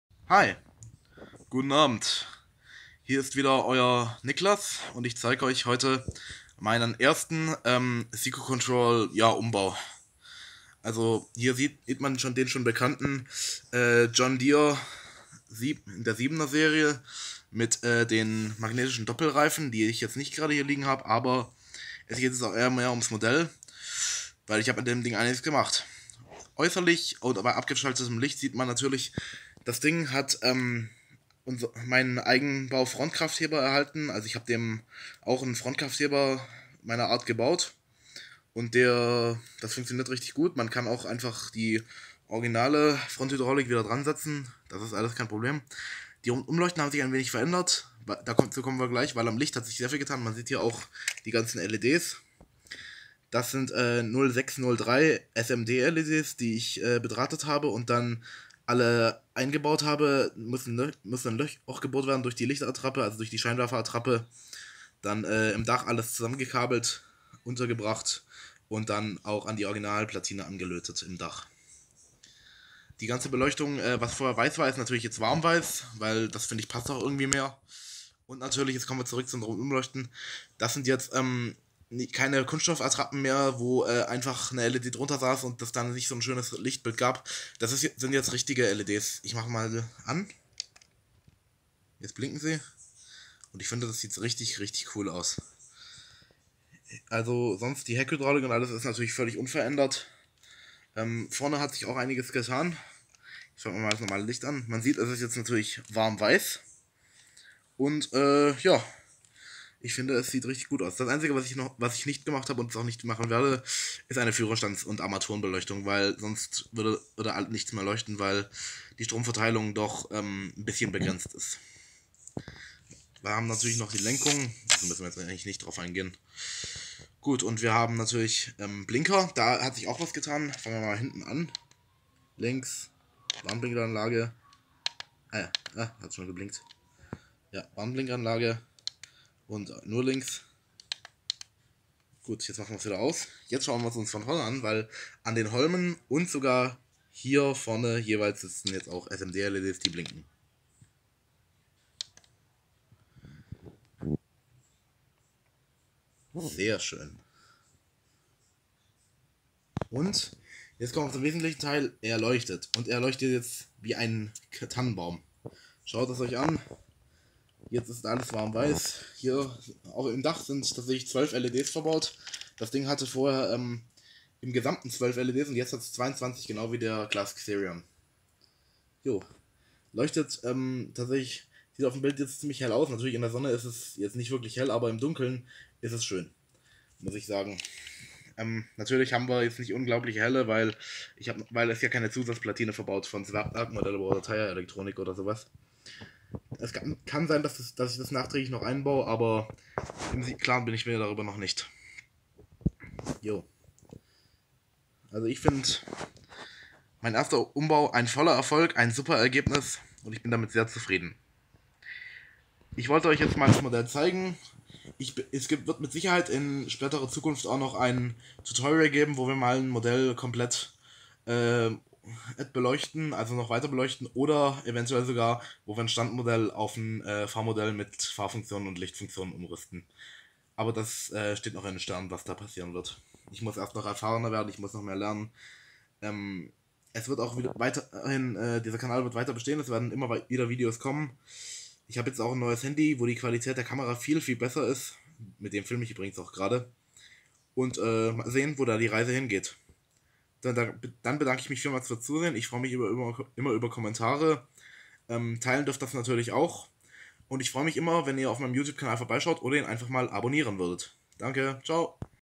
Siku Control Bluetooth John Deere 7er mit komplettem Lichtumbau
Hier seht ihr meinen ersten Siku Control Umbau in Action. Wunderschönes Modell :)

SMD Led's sind von eBay.

Kupferlackdraht bekommt man auf eBay und Amazon.

Die 3mm LED's sind von einer alten Lichterkette, bekommt man aber auch bei eBay.

Die erwähnten Zusatzplatinen und Licht/Servo Module bekommt ihr im Shop von Modell Trecker Tuning.